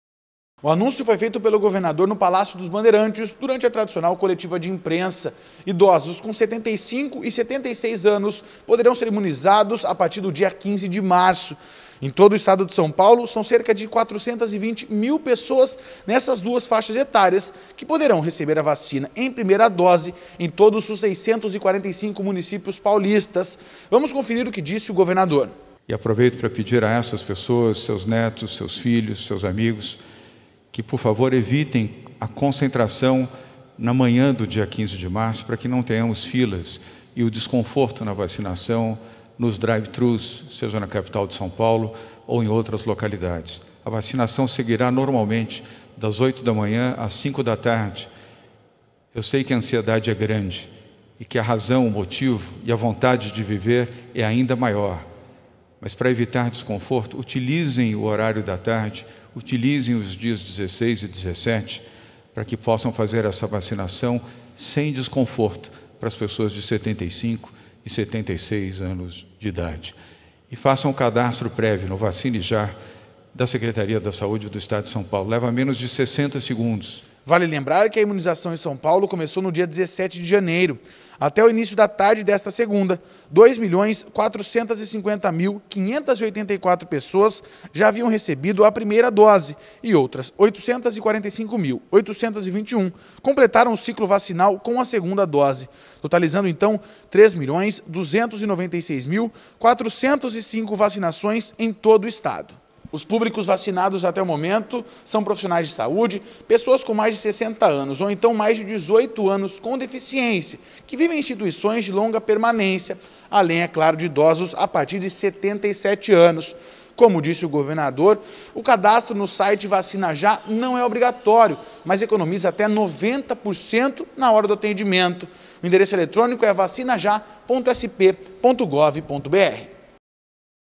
Governador João Dória anuncia nova etapa de imunização
Idosos com 75 e 76 anos começarão a ser vacinados no dia 15 de março.